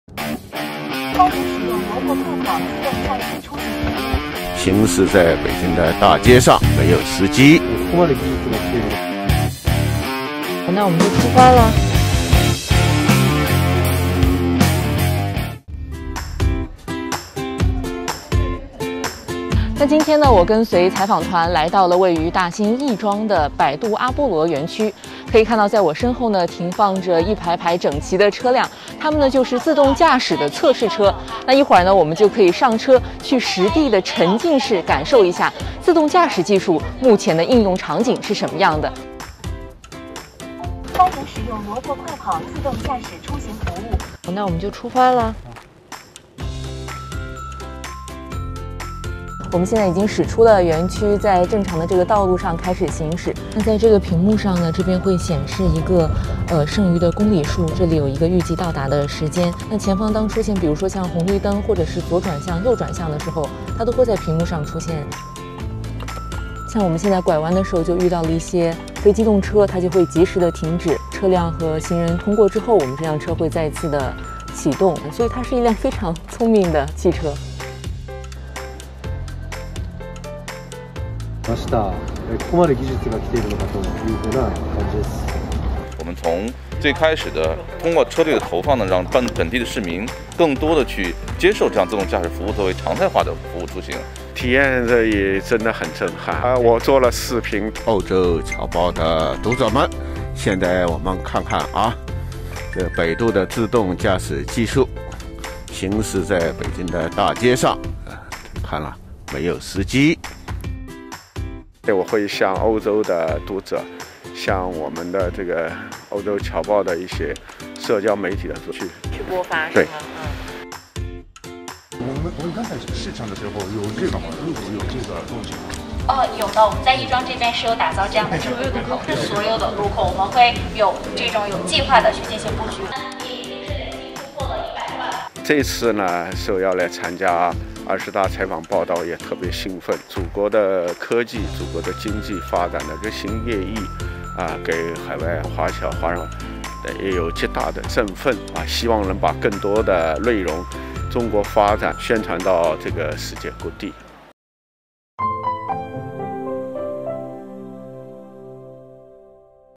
境内外记者体验自动驾驶 直言“很震撼”！
10月14日，在中共二十大召开前夕，60余位境内外记者走进位于北京大兴区的百度阿波罗科技园，沉浸式体验自动驾驶等科技应用场景。不少境外媒体记者上车体验，并用镜头记录下自己的所见所闻。（郎佳慧 单璐）